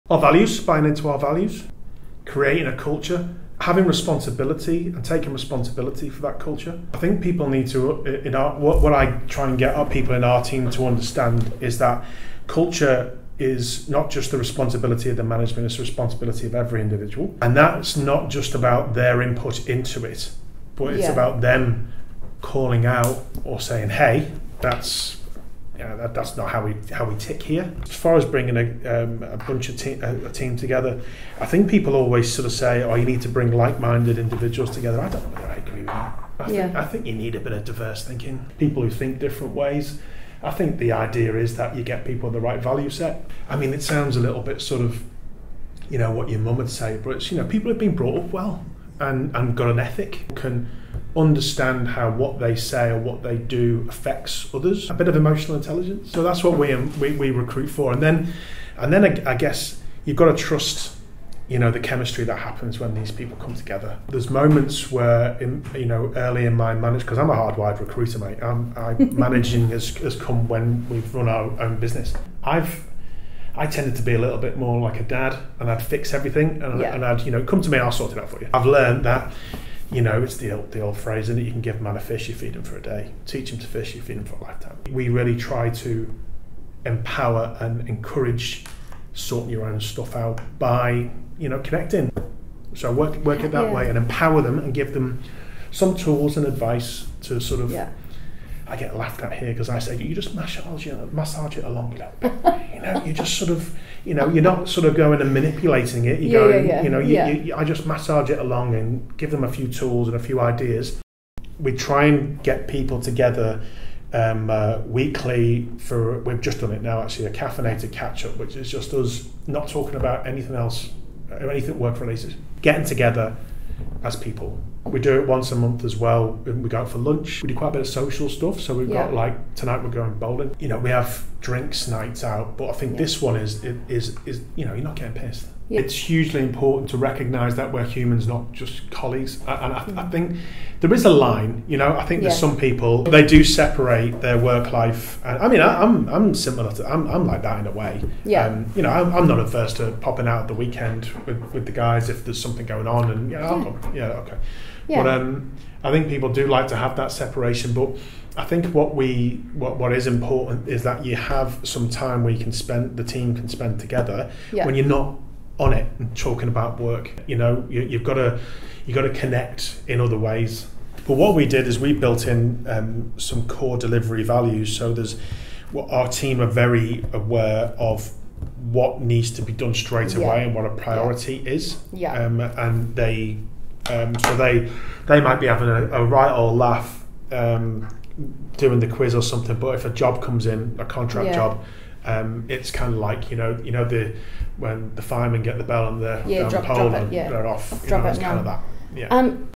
Humans of Younity: Mark's insights into creating a diverse, strong and happy recruitment team
We sat down with Mark Warren, Manager of our Auckland team (and one of the four founders of Younity) to hear more about how he builds strong team cohesion and connection, his love for diversity in his team, and the lessons he's learnt in team management.

Want to read the full interview?  head over to our News section to read more.